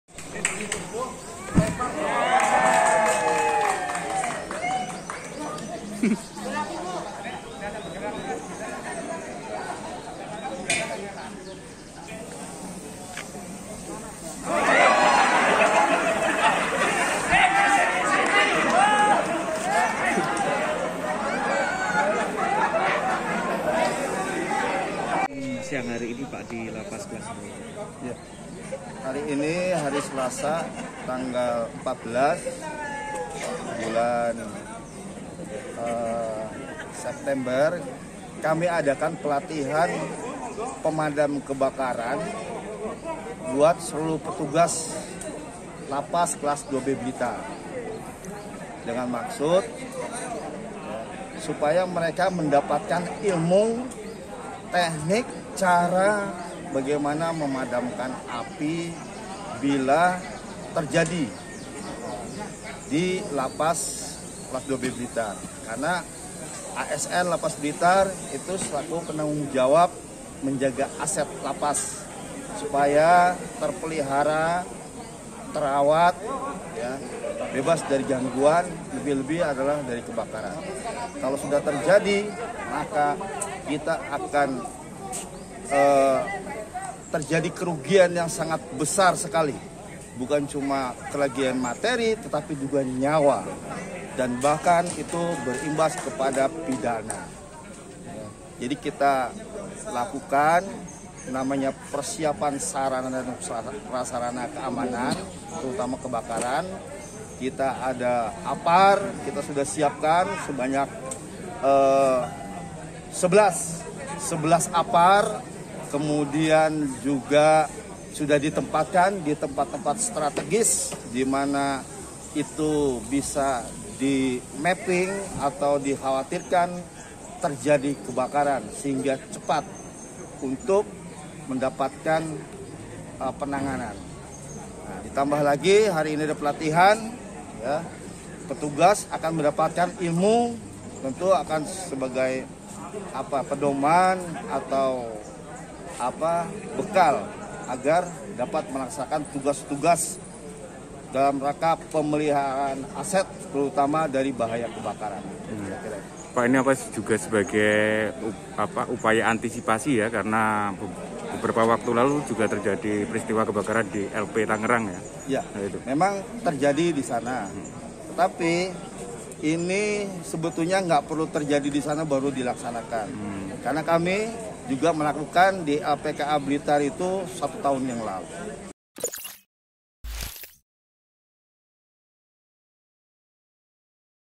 PETUGAS LP BLITAR DILATIH PENANGGULANGAN KEBAKARAN
Video & reporter : Samsul Hadi
Editing: nang

SURYA.CO.ID, SURYA - Lembaga Pemasyarakatan (LP) Kelas IIB Blitar menggelar pelatihan penanggulangan kebakaran di LP, Selasa (14/9/2021).

Pelatihan penanggulangan kebakaran itu sebagai langkah antisipasi pasca terjadinya kebakatan di LP Tangerang yang menewaskan 45 Napi.

Pelatihan penanggulangan kebakaran diikuti para petugas LP Kelas IIB Blitar.

Para petugas LP mendapat materi soal penanggulangan kebakaran dari petugas UPT Pemadam Kebakaran Kota Blitar.

Para petugas LP juga ikut mempraktikan cara pemadaman api menggunakan apar dan kain basah.

"Pelatihan penanggulangan kebakaran diikuti semua petugas LP Kelas IIB Blitar," Plt Kepala LP Kelas IIB Blitar, Tatang Suherman.

Tatang mengatakan kegiatan itu untuk melatih keterampilan petugas LP dalam penanggulangan kebakaran.

Karena para petugas LP mempunyai tanggung jawab memelihara dan merawat aset dari gangguan lebih-lebih peristiwa kebakaran di dalam LP.

"Peristiwa kebakaran dapat menimbulkan kerugian sangat besar baik secara materiil dan jiwa," ujarnya.

Dikatakannya, LP juga sudah menyiapkan sarana dan prasarana untuk penanggulangan kebakaran.

LP sudah memiliki 11 apar yang diletakkan di beberapa tempat strategis.

"Ditambah lagi hari ini ada pelatihan sebagai bekal para petugas untuk antisipasi penanggulangan kebakaran," katanya.

Menurutnya, pelatihan itu juga sebagai langkah antisipasi pasca terjadi kebakaran di LP Tangerang.

"Memang terjadi kebakaran di sana (LP Tangerang), tapi sebetulnya pelatihan ini nggak perlu menunggu terjadi baru dilakukan. Kami sudah melaksanakan di LPKA setahun lalu," ujarnya.

Dikatakannya, pelatihan penanggulangan kebakaran ini sudah masuk perencanaan di LP Kelas IIB Blitar.

"Jadi ini sudah perencanaan. Kami minta dipercepat segera koordinasi untuk melaksanakan pelatihan," ujarnya.

Sekadar diketahui, saat ini, jumlah Napi di LP Kelas IIB Blitar sebanyak 574 orang.

Sedang kapasitas LP Kelas IIB Blitar hanya 132 orang. Terjadi over kapasitas di LP Kelas IIB Blitar.

"Jumlah petugas kami hanya 68 orang," katanya.